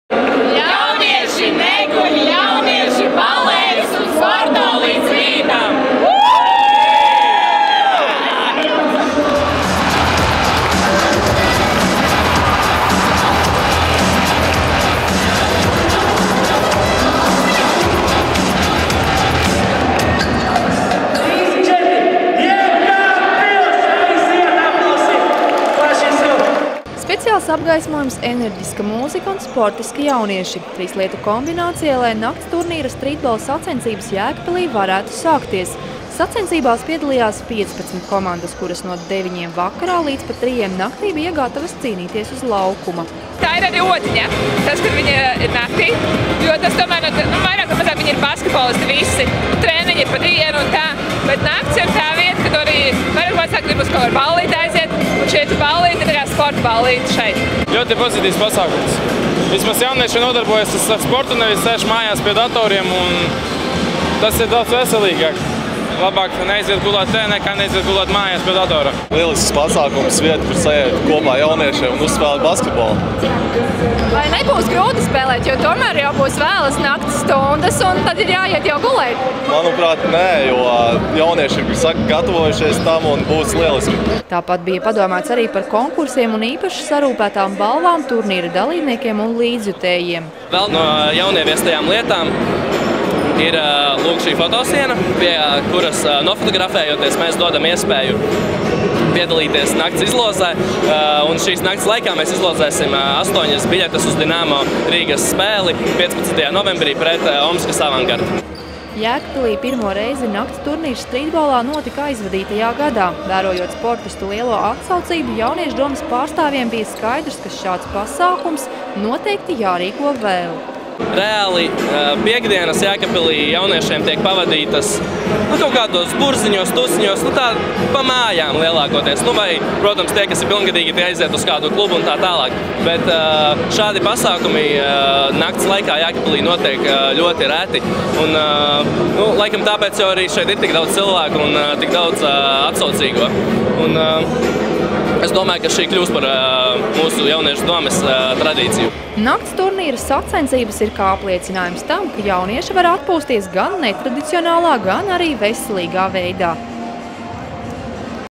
Jēkabpils nakts turnīrs strītbolā
Speciāls apgaismojums, enerģiska mūzika un sportiski jaunieši - trīs lietu kombinācija, lai nakts turnīra sacensības Jēkabpilī varētu sākties. Sacensībās piedalījās piecpadsmit komandas, kuras no deviņiem vakarā līdz pat trijiem naktī bija gatavas cīnīties uz laukuma.